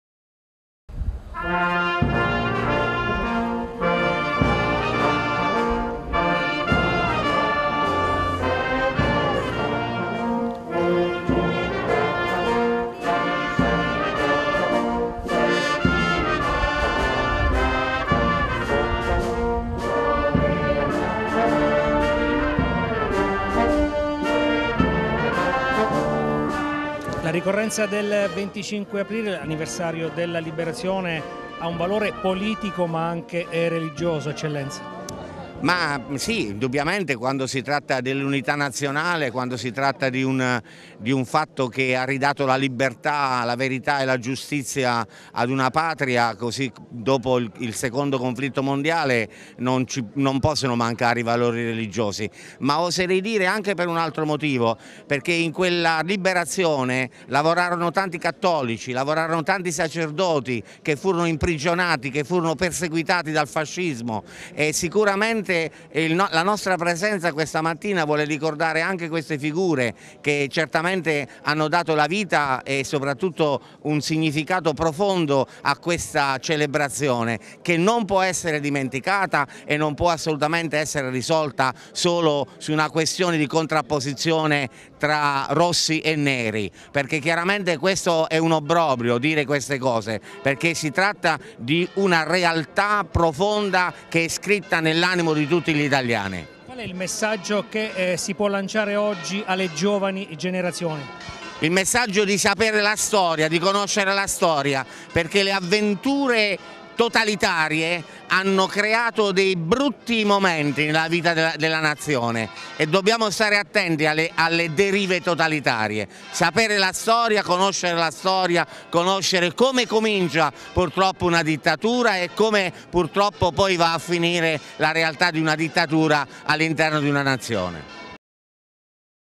Festa della Liberazione. Pescara ricorda il 25 aprile
Anche a Pescara, come in altri centri abruzzesi, si sono tenute le iniziative per celebrare la ricorrenza del 25 aprile. In Piazza Garibaldi deposta una corona d'alloro in memoria delle vittime del secondo conflitto mondiale